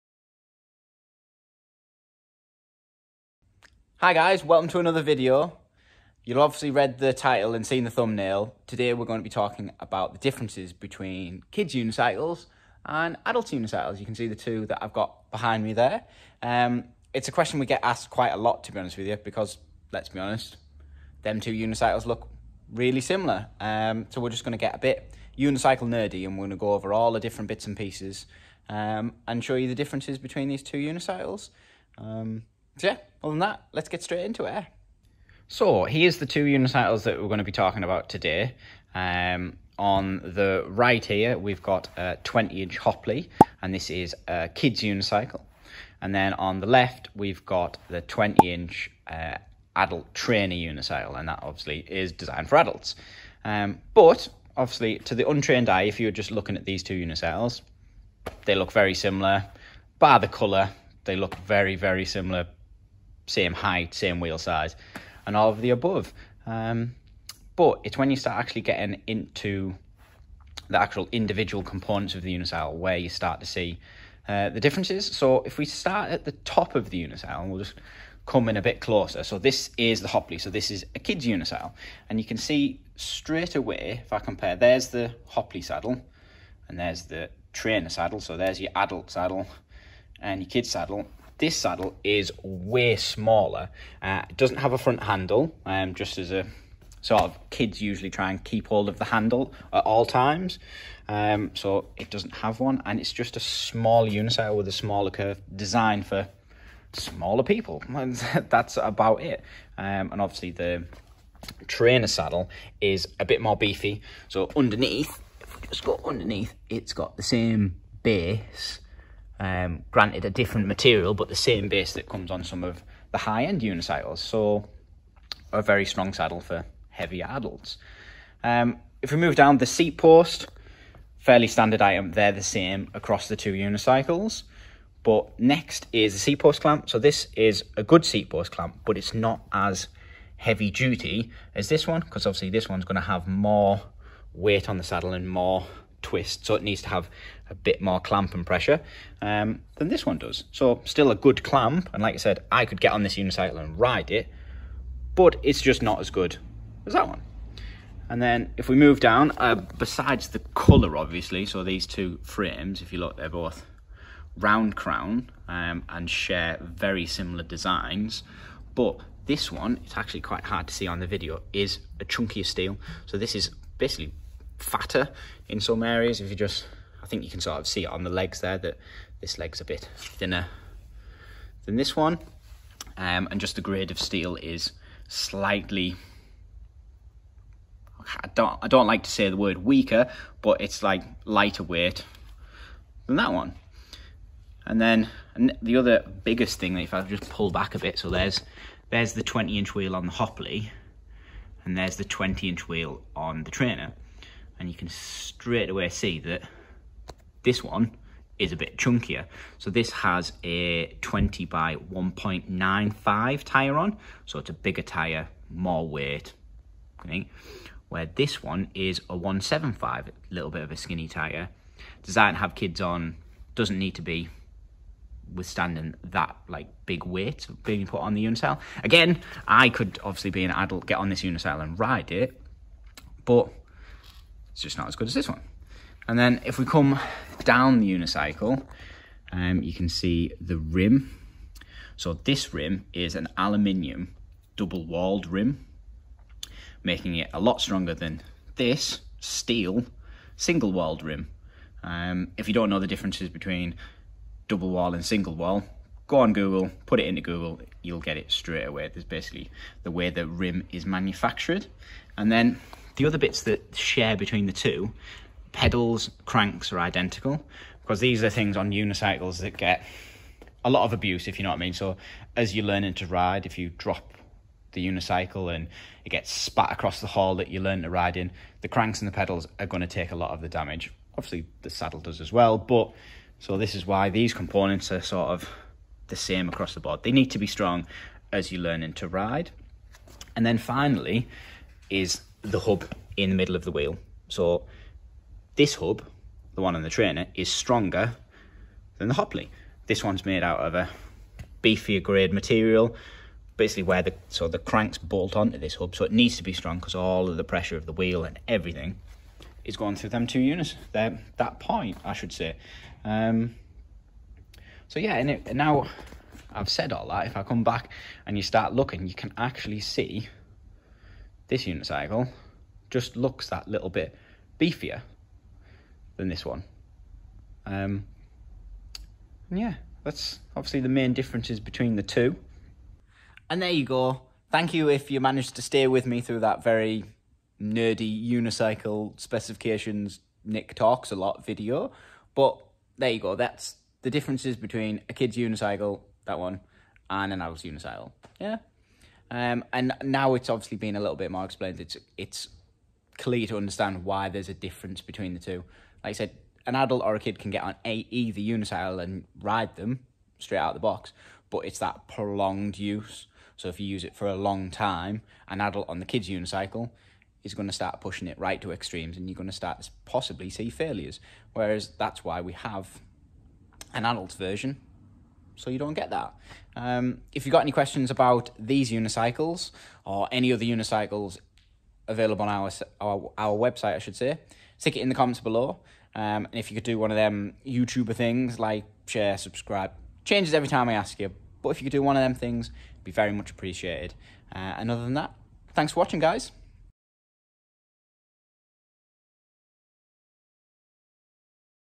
Differences Between Kids and Adults Unicycles?
Today’s video we look at the differences between a Kids Unicycle and an Adults Unicycle, because let’s be honest they look the same. We go through all the components and get really “Unicycle Nerdy” comparing the two cycles.